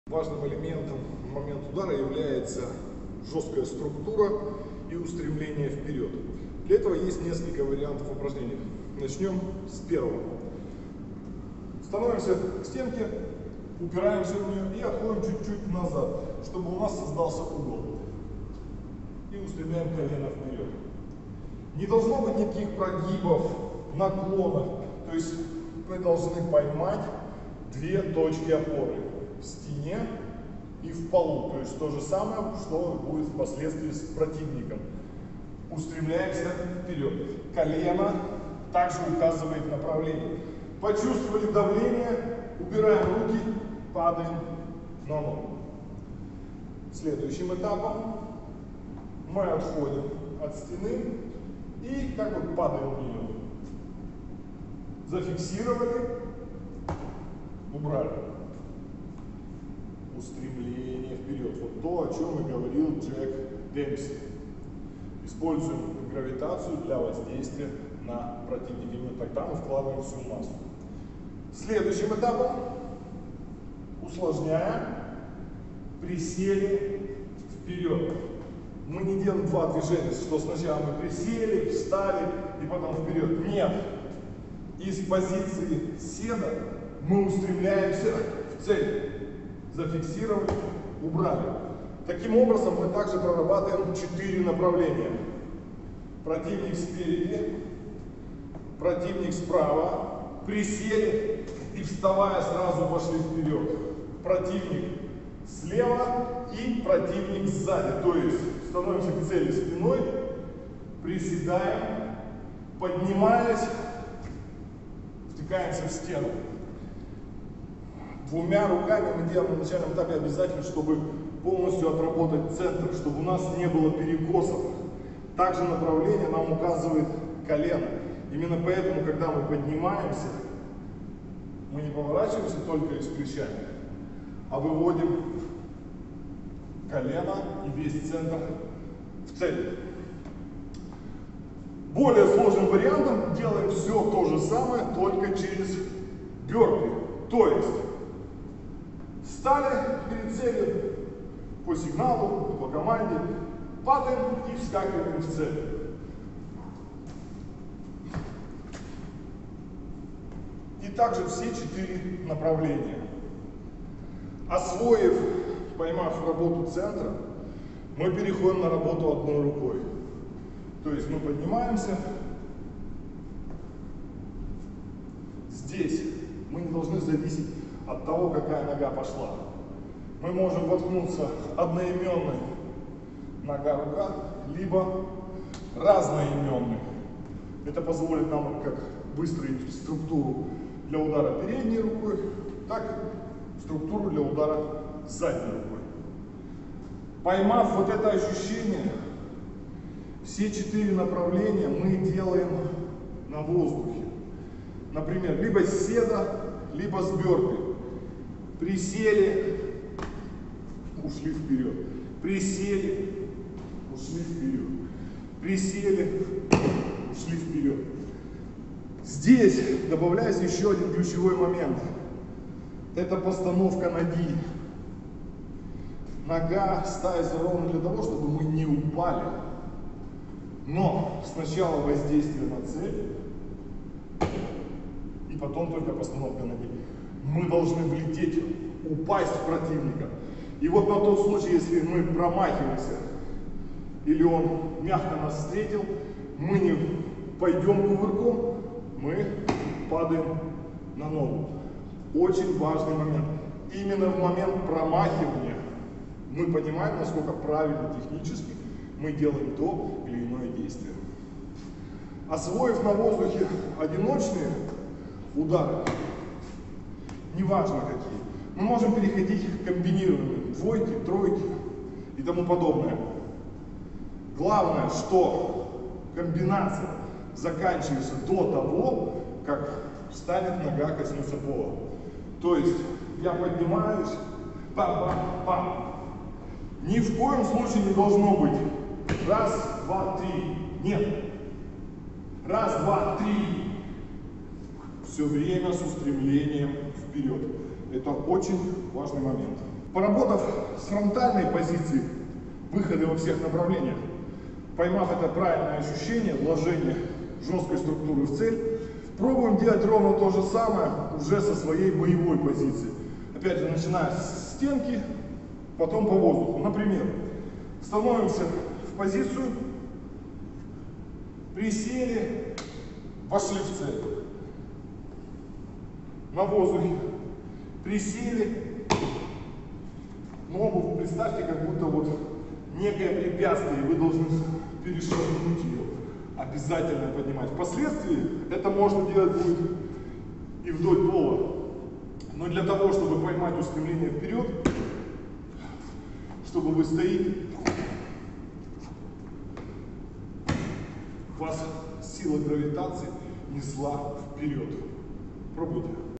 Атака Структура и устремление.
Важным в атаке является вложение массы в удар. Мало это понимать, надо ещё и делать это на уровне рефлексов. Атака - всегда направлена вперёд. Несколько подводящих, подсобных упражнений для формирования правильного устремления в атаке и вложения всей массы тела. В своё время об этом писал Джек Демпси в своей книге и описывал -  падающий шаг. При наработке для спортивного Ножевого боя - просто добавляем в руку макет ножа и выполняем те же действия. Однако стоит учитывать, что увеличивается нагрузка на луче-запястный сустав. Или же просто добавить клинок в фазе отработки на воздухе, без внешней опоры.
Как базовый формат работы всего тела - подходит для наработки как в рукопашных системах, так и фехтовальных.